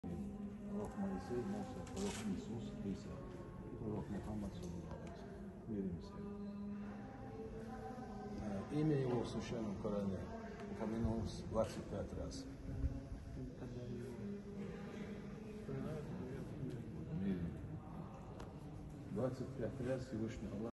В мечети Нашего Президента встретил уважаемый Муфтий ЧР Шейх Салах-Хаджи Межиев.
Владимир Путин в Грозном в сопровождении Рамзана Кадырова посетил новую мечеть имени пророка Исы, сообщают журналисты президентского пула.

Vladimir Putin in Grozny, accompanied by Ramzan Kadyrov, visited the new mosque named after the Prophet Isa , report journalists from the presidential pool.